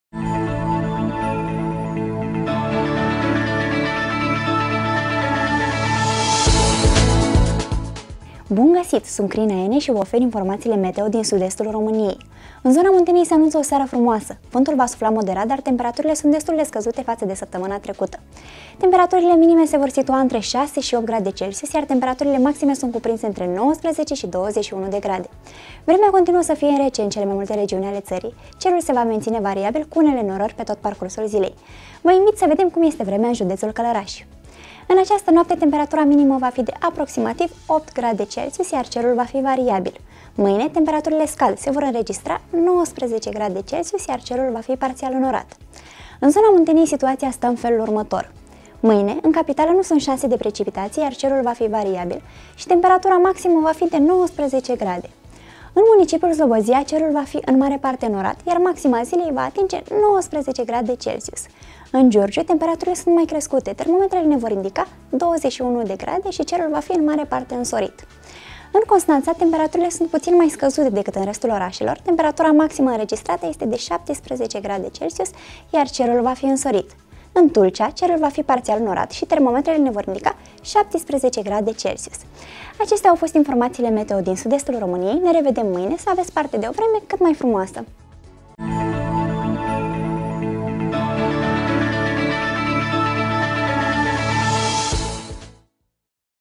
CANAL SUD / Vremea 13 octombrie 2022
În zona Munteniei se anunță o seară frumoasă, vântul va sufla moderat, dar temperaturile sunt destul de scăzute față de săptămâna trecută.
Temperaturile minime se vor situa între 6 și 8 grade Celsius, iar temperaturile maxime sunt cuprinse între 19 Și 21 de grade.
Vremea continuă să fie rece în cele mai multe regiuni ale țării. Cerul se va menține variabil, cu unele înnorări pe tot parcursul zilei.